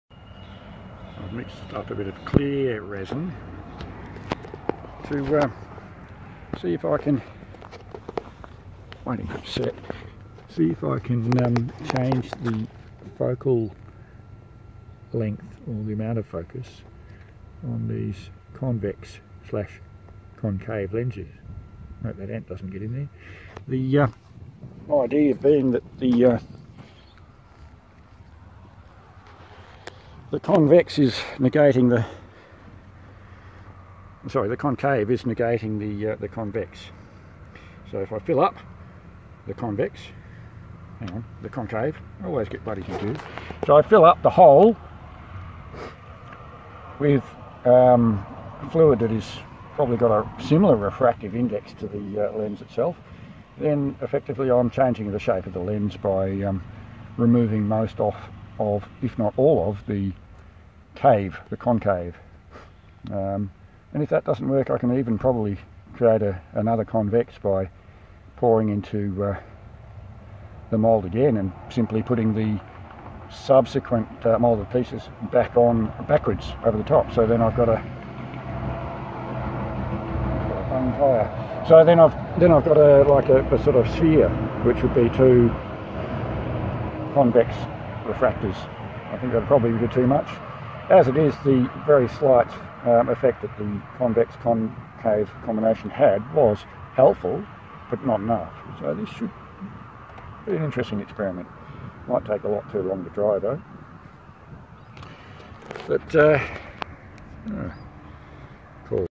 Modifying spotlight lenses
Whilst in Dingo, charging up, and mucking about with my lights, again.